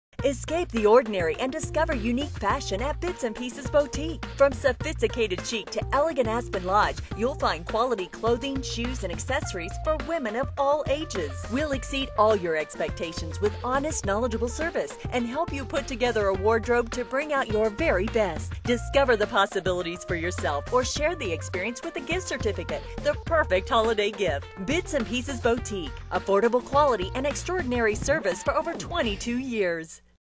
Bits & Pieces: Our First Holiday Cable Commercial
Hi Everyone,
We are so excited for the end of 2009 as it is the first time in the 22 year history of our store that we are airing cable television commercials. Thank you to all who have supported us in the past and a very warm welcome to those of you who are just making an exciting discovery of Bits & Pieces Boutique. Welcome aboard!